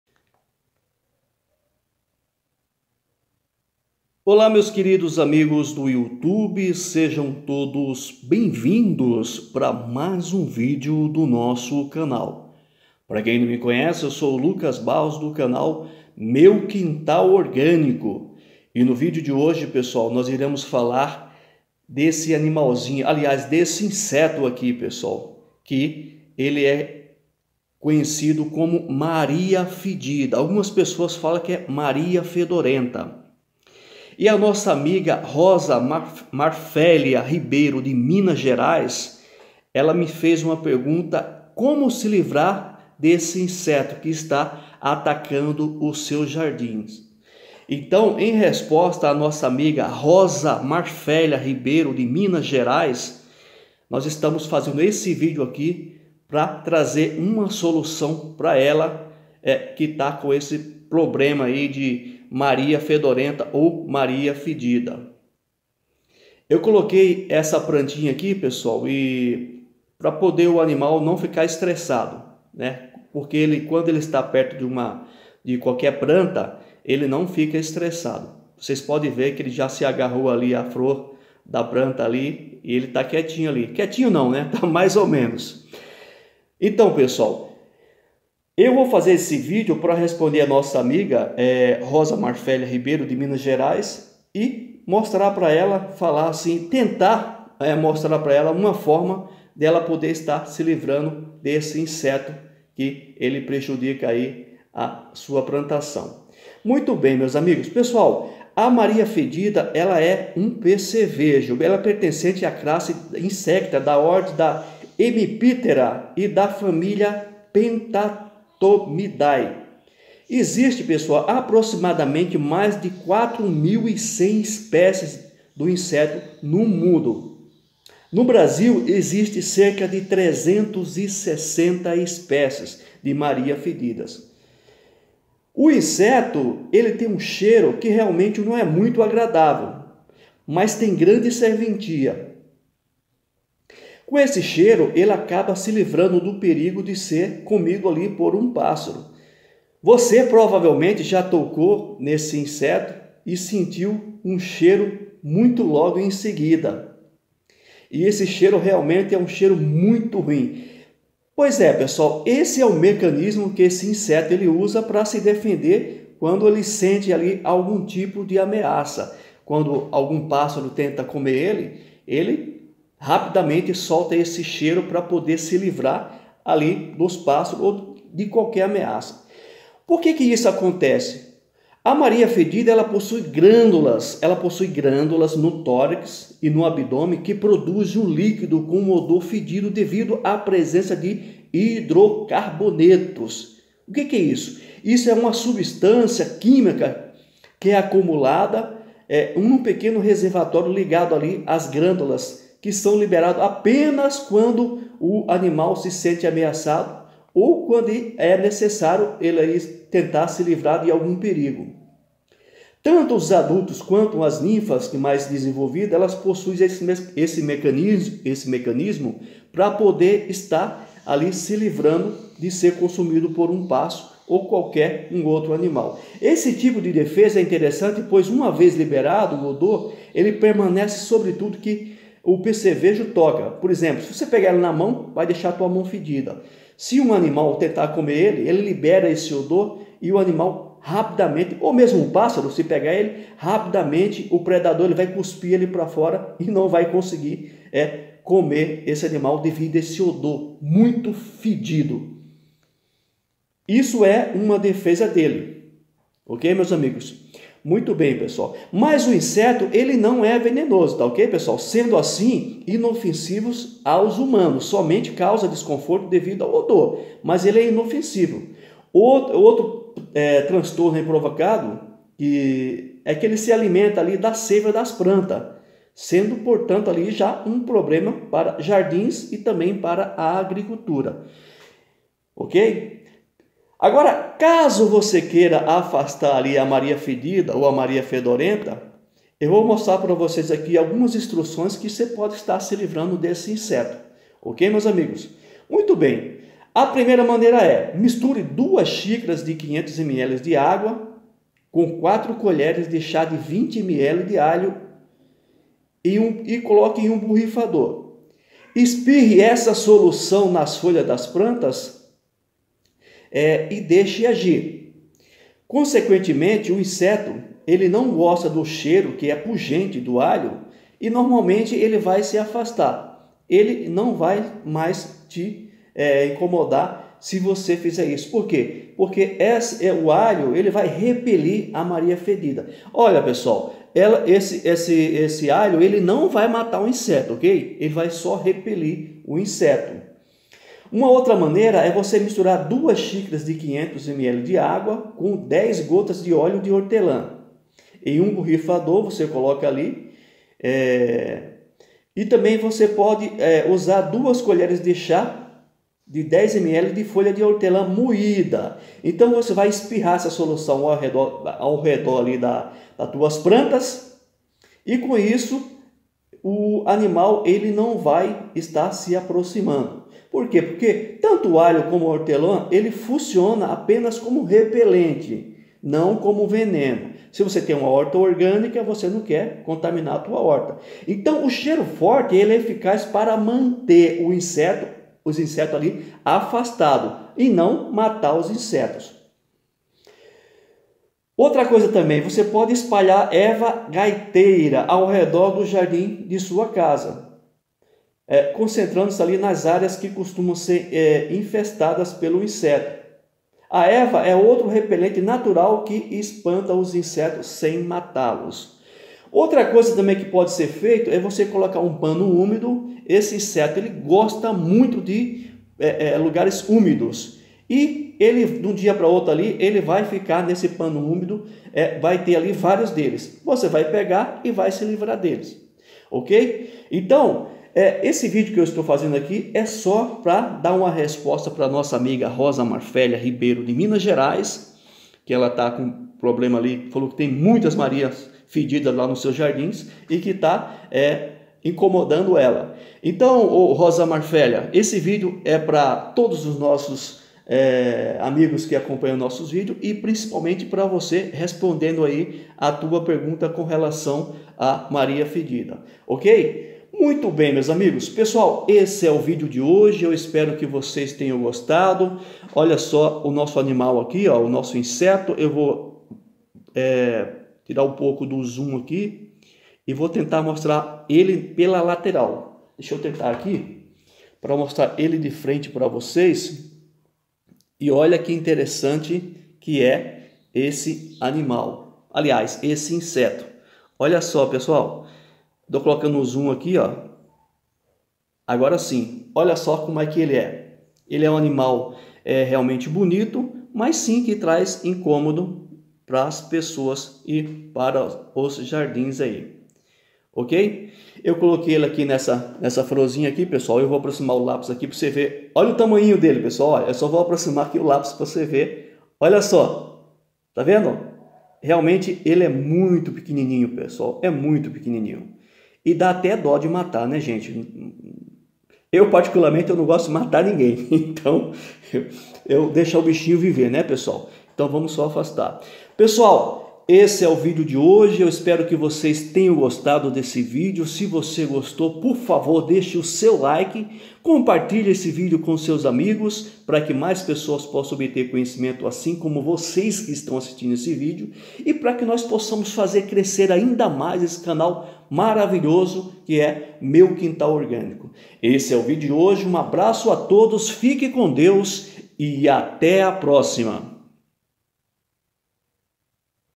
Como se livrar do inseto MARIA FEDIDA
Nesse vídeo nós vamos ensinar algumas maneiras de como está se livrando do inseto maria f que ataca tanto plantações como os jardins. Isso de maneira orgânica.